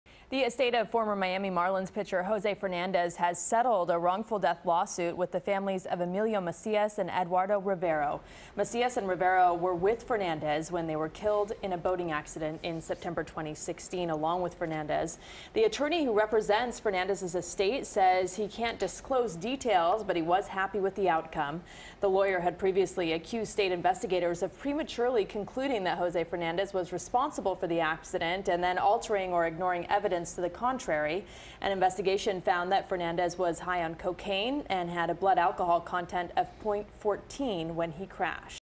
Lawsuit Settled In Deadly Boat Crash Of Late Marlins Pitcher Jose Fernandez
A wrongful death lawsuit filed in the case involving the late Miami Marlins pitcher Jose Fernandez has been settled.